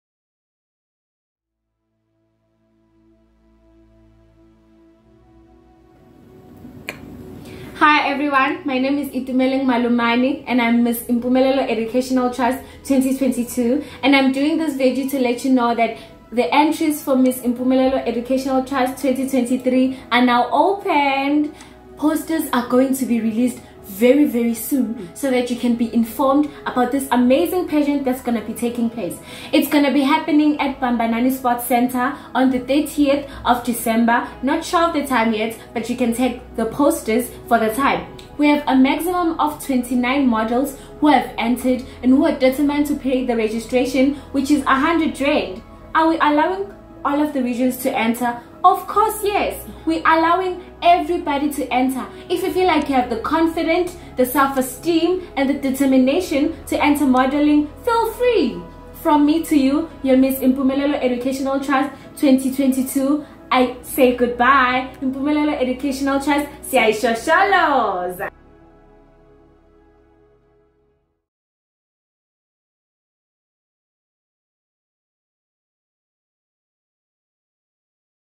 MISS IET 2023 ENTRIES ARE OPEN//SOUTH African Youtuber
THIS VIDEO IS ABOUT THE ENTRIES ON THE IET BEAUTY PAGEANTS.
Mpumalanga Pageants.
DIY's.
# Podcast